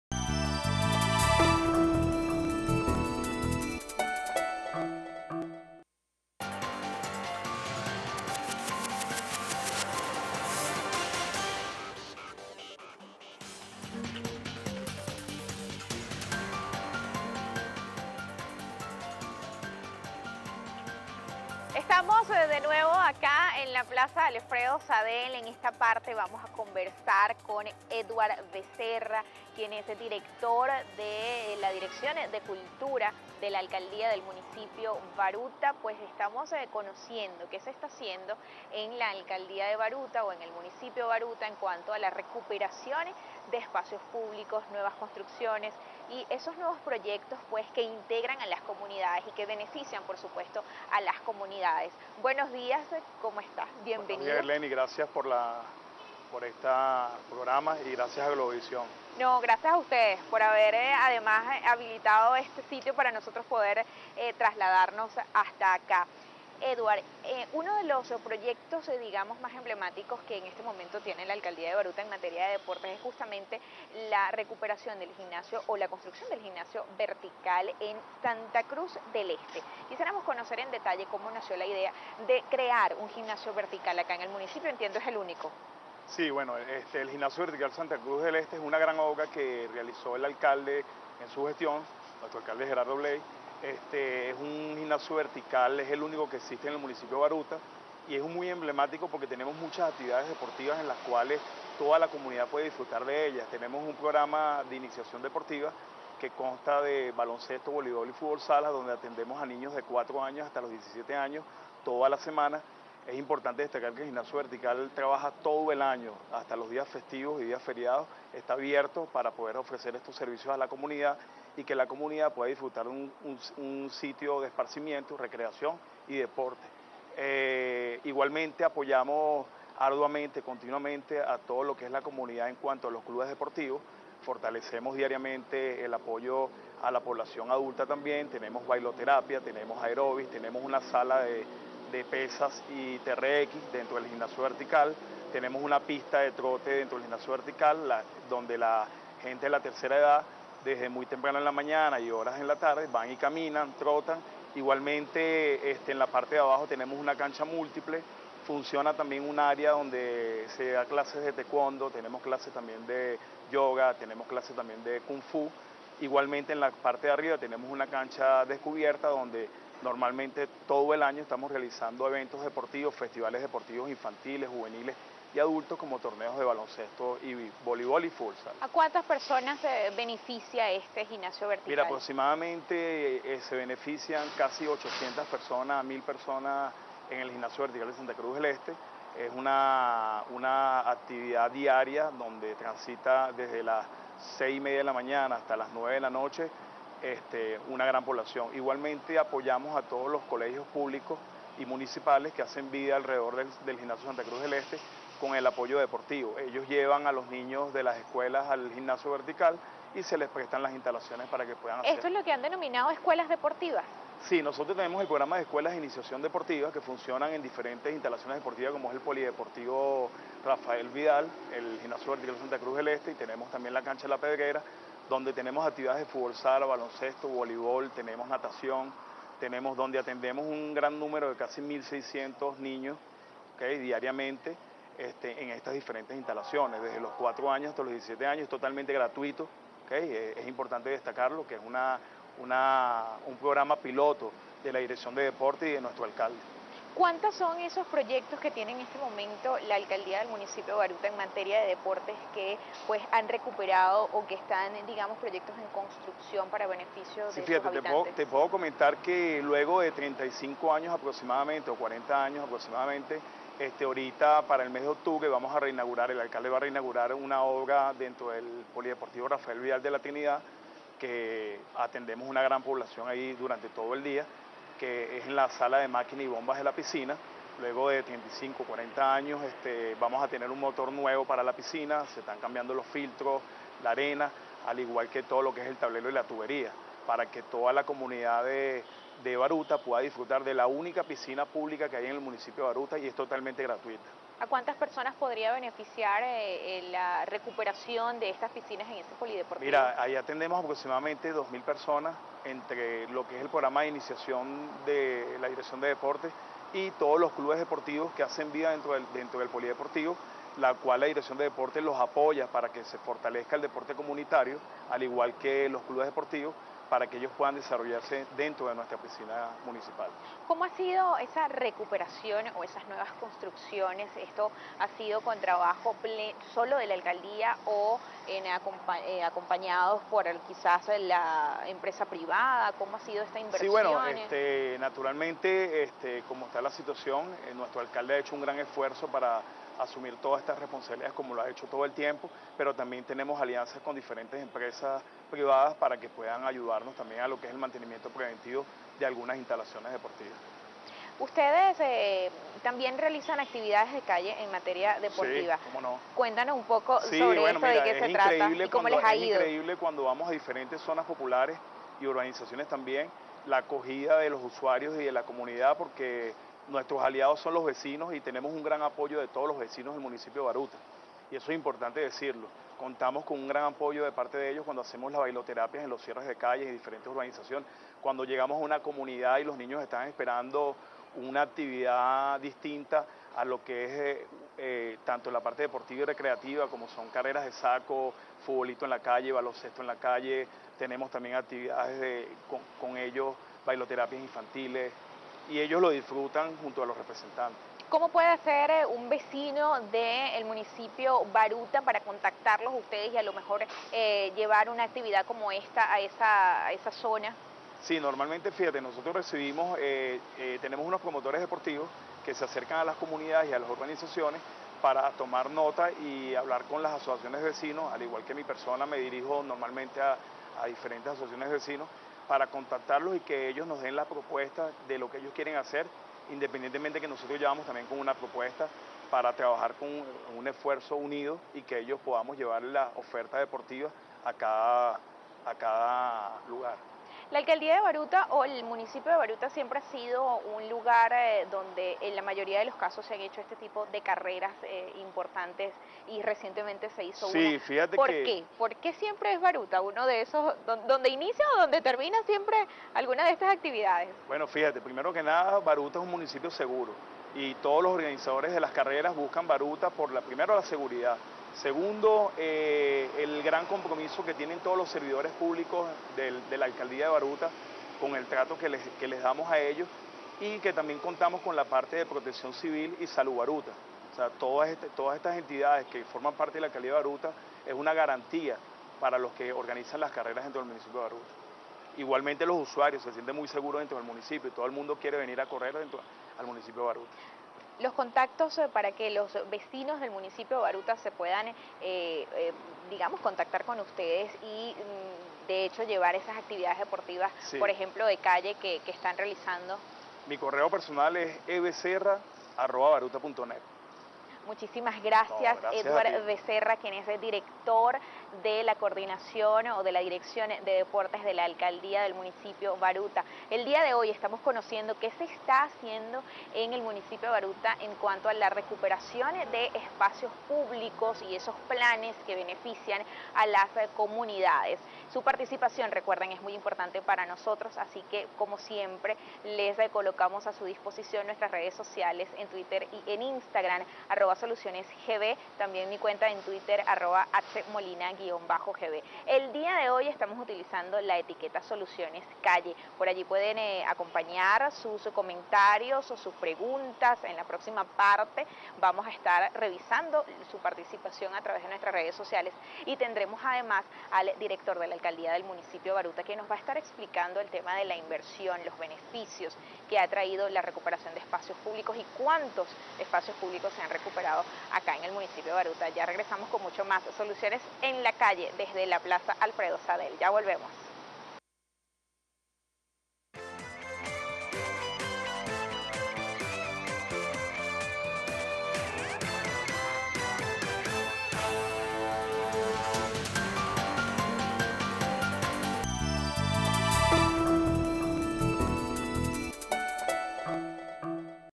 Estos son los espacios que ofrece Baruta para la recreación y esparcimiento
El municipio cuenta con el Gimnasio Vertical de Santa Cruz del Este y el Polideportivo Rafael Vidal de La Trinidad con programas de iniciación deportiva de manera gratuita, los cuales están a disposición de los habitantes todos los días, entre otro más.